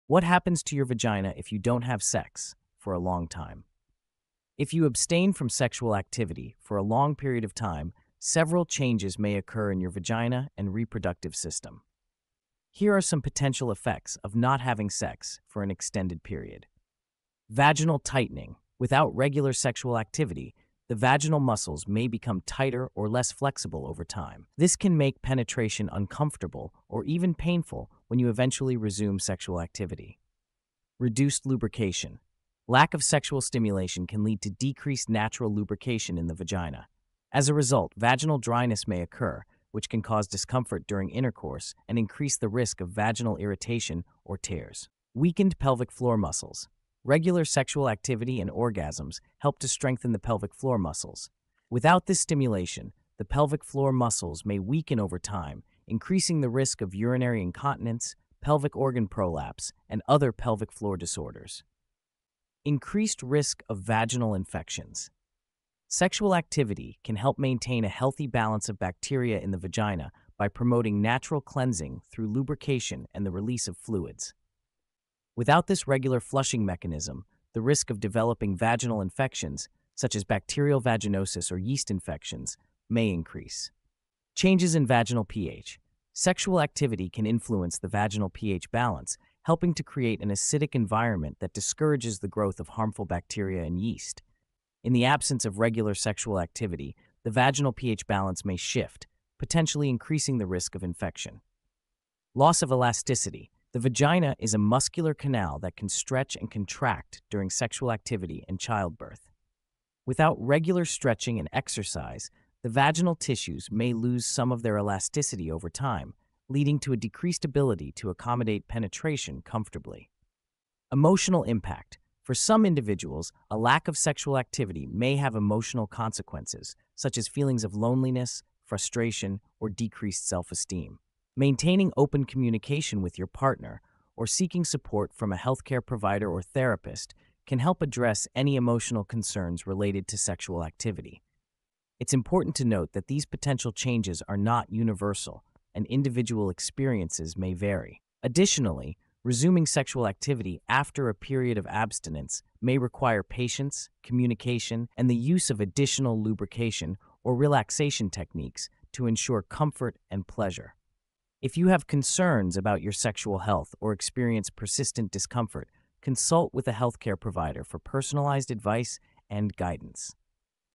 Warning: Your Vagina Without Sex
Transcript:
(00:00) what happens to your vagina if you don't have sex for a long time if you abstain from sexual activity for a long period of time several changes may occur in your vagina and reproductive system here are some potential effects of not having sex for an extended period vaginal tightening without regular sexual activity the vaginal muscles may become tighter or less flexible over time this can make penetration uncomfortable or even painful when you eventually resume sexual activity reduced lubrication lack of sexual
(00:33) stimulation can lead to decreased natural lubrication in the vagina as a result vaginal dryness may occur which can cause discomfort during intercourse and increase the risk of vaginal irritation or tears weakened pelvic floor muscles regular sexual activity in orgasms help to strengthen the pelvic floor muscles without this stimulation the pelvic floor muscles May weaken over time increasing the risk of urinary incontinence pelvic organ prolapse and other pelvic floor disorders increased risk of vaginal infections sexual activity can help
(01:07) maintain a healthy balance of bacteria in the vagina by promoting natural cleansing through lubrication and the release of fluids without this regular flushing mechanism the risk of developing vaginal infections such as bacterial vaginosis or yeast infections may increase changes in vaginal pH sexual activity can influence the vaginal pH balance helping to create create an acidic environment that discourages the growth of harmful bacteria and yeast in the absence of regular sexual activity the vaginal pH balance may shift potentially increasing
(01:40) the risk of infection loss of elasticity the vagina is a muscular Canal that can stretch and contract during sexual activity and childbirth without regular stretching and exercise the vaginal tissues may lose some of their elasticity over time leading to a decreased ability to accommodate penetration comfortably emotional impact for some individuals a lack of sexual activity may have emotional consequences such as feelings of loneliness frustration or decreased self-esteem maintaining open communication with your partner or
(02:14) seeking support from a health care provider or therapist can help address any emotional concerns related to sexual activity it's important to note that these potential changes are not Universal and individual experiences may vary additionally resuming sexual ual activity after a period of abstinence may require patience communication and the use of additional lubrication or relaxation techniques to ensure comfort and pleasure if you have concerns about your sexual health or experience persistent discomfort consult with a
(02:47) healthc care provider for personalized advice and guidance